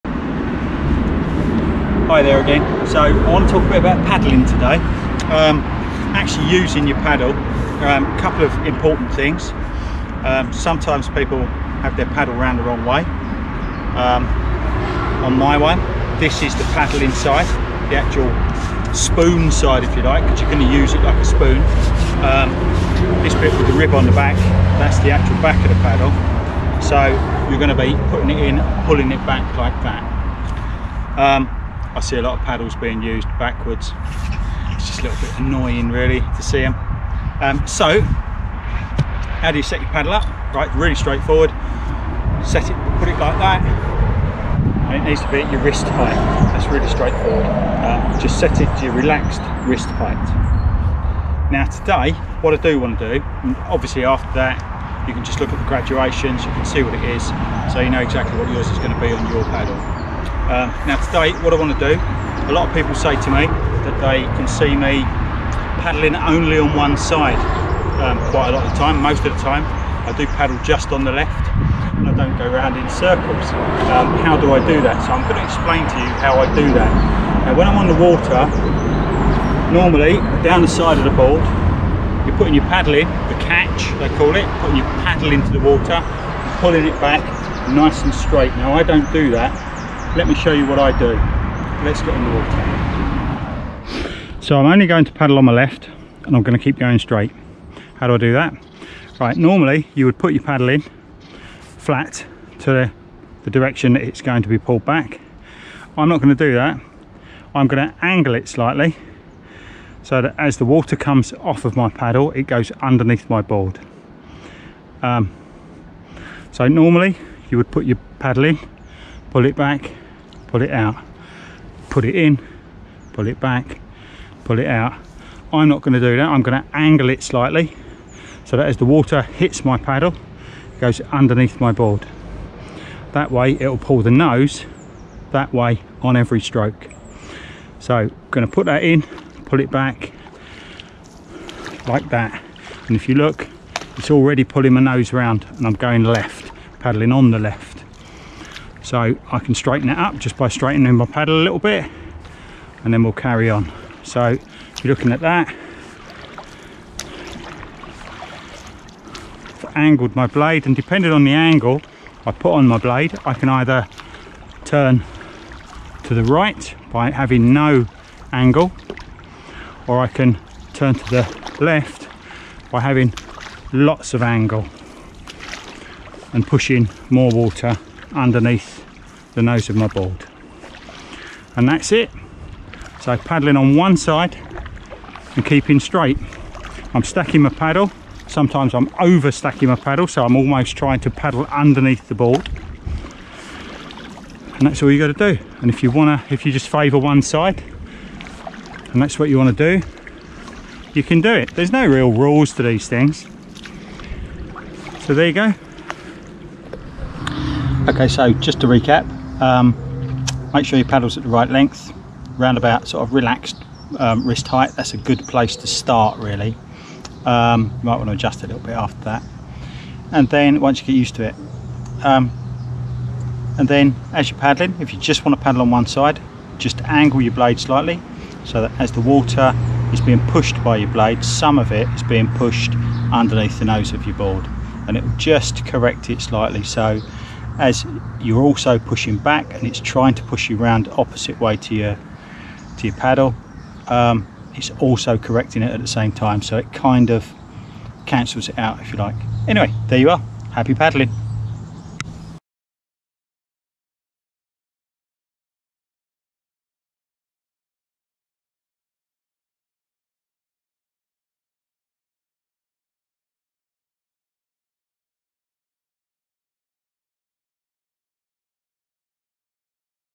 Paddle Boarding - Paddling Straight (only on one side)
A lot of people ask me how I stay straight when I mostly only paddle on the left... here's how I do it... Happy Paddling!

Main points

Set your paddle length
Paddle face
Blade angle
Over stacking